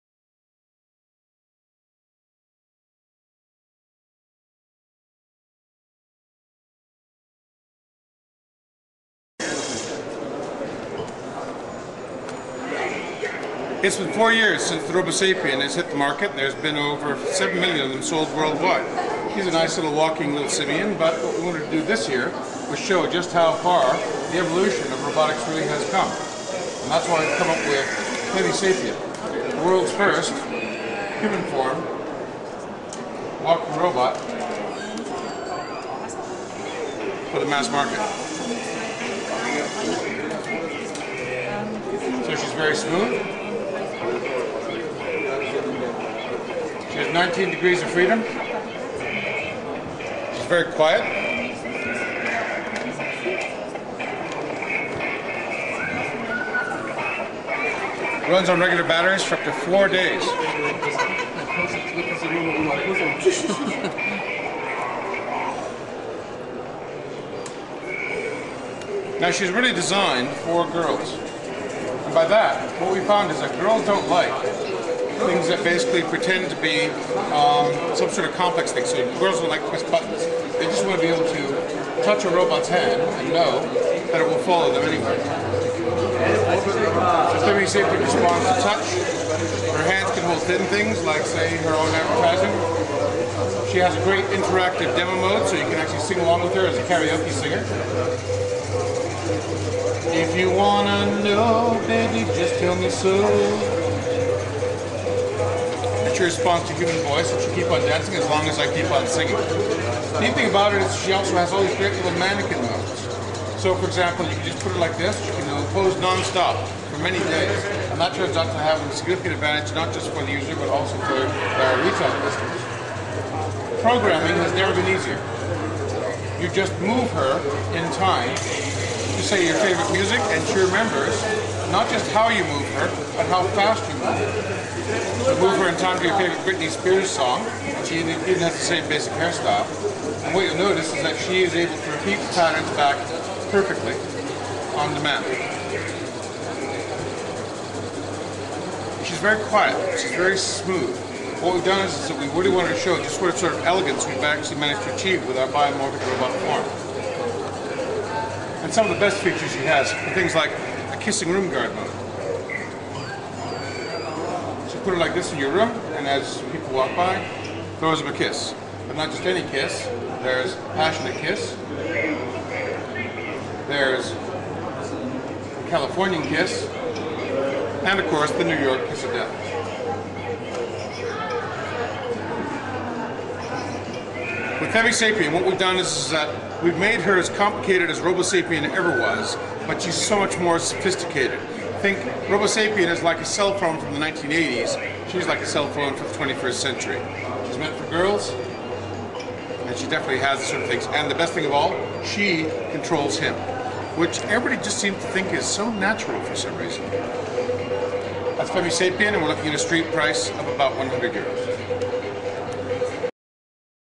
MARK TILDEN MUESTRA A FEMISAPIEN NUEVO ROBOT DE WOW WEE
MARK TILDEN EL CREADOR DE LOS FAMOSOS ROBOTS ROBOSAPIEN, NOS MUESTRA AL EQUIPO DE JUGUETRONICA EL NUEVO ROBOT FEMENINO DE WOW WEE FEMISAPIEN. MARK TILDEN THE DESIGNER OF THE FAMOUS ROBOSAPIEN ROBOTS SHOWS TO THE JUGUETRONICA CREW THE NEW FEMISAPIEN FEMALE ROBOT.